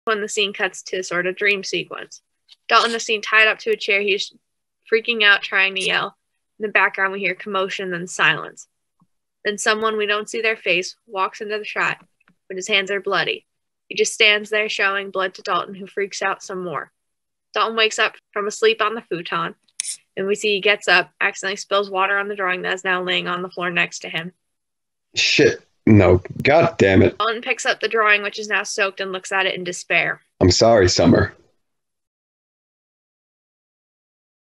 Fracture Episode One: Bandana (Table Read)
Blue-J Studios presents a sneak peak into the table read of Fracture Episode One.